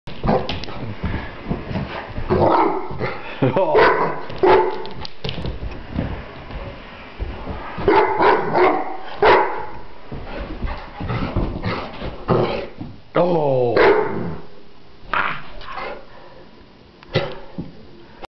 Lily wants to play
1 year old English Mastiff wants to play